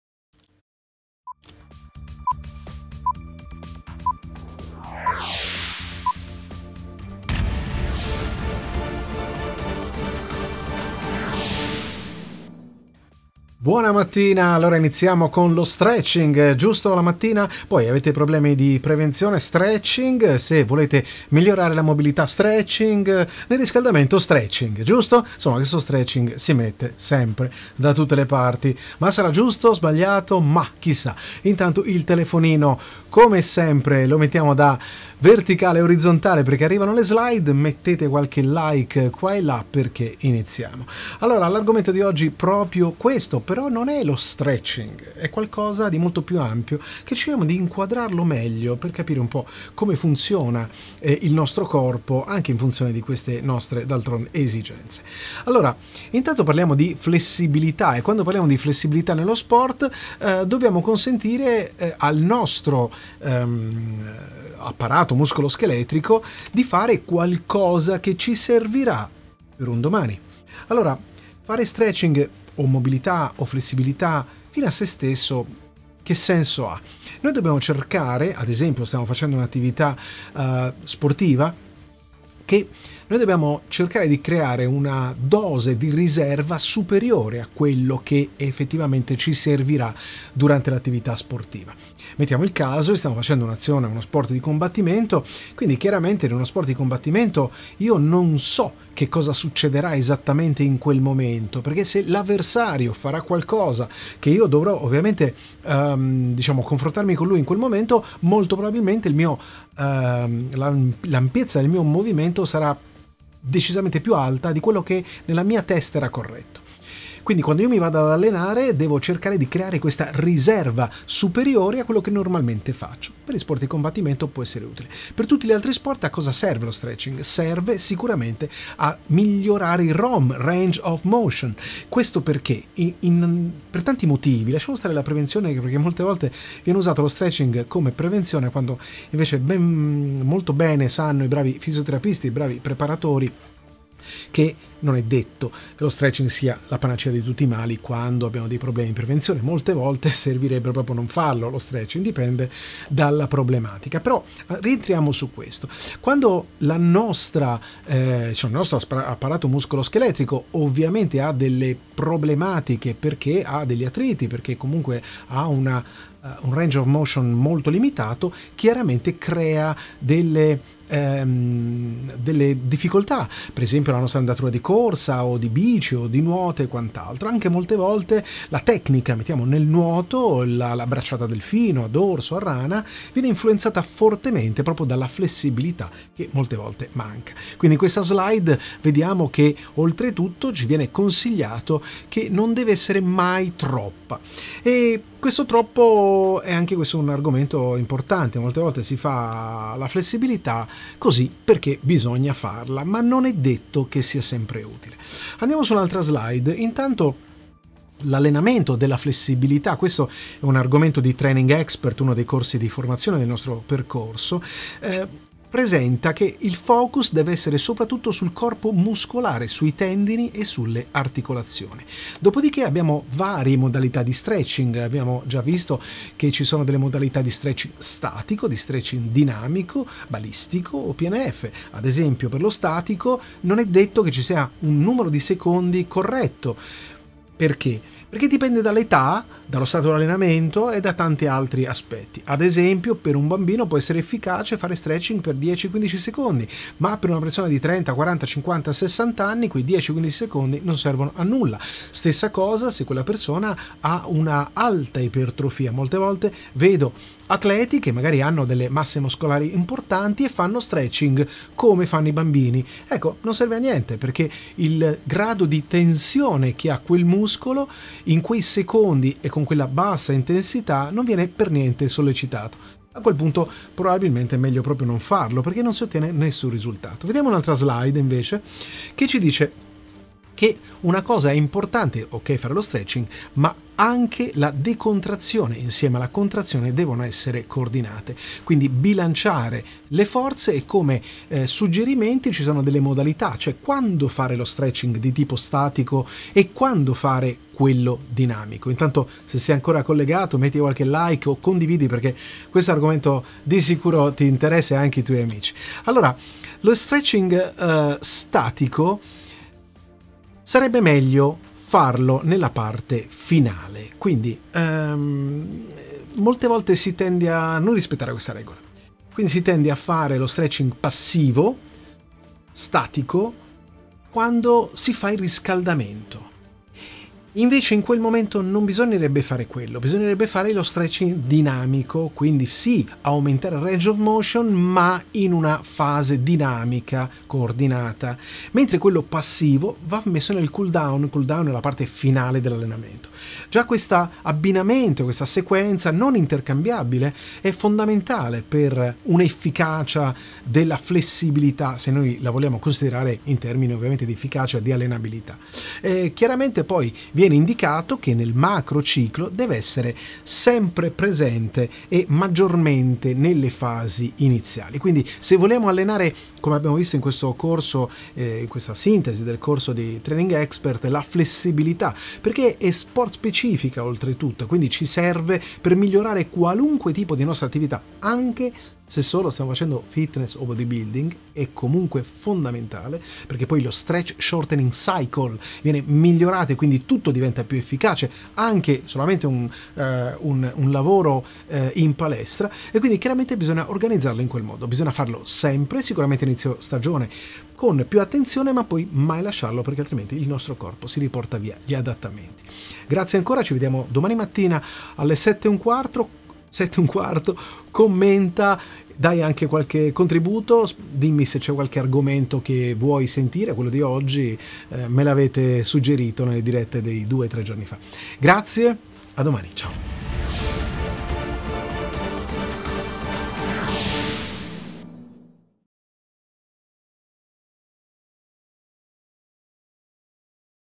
Ma lo Stretching a cosa serve?
Lo stretching serve? E perchè bisogna farlo? Con quale modalità ed in che momento della preparazione? Nella'llenamento sportivo o funzionale o di bodybuilding va inserito?
Tutte tematiche che vengono affrontate da Gian Mario Migliaccio in questa LIVE.

Canale video per conoscere le novità su Sport & Nutrizione basate su evidenze scientifiche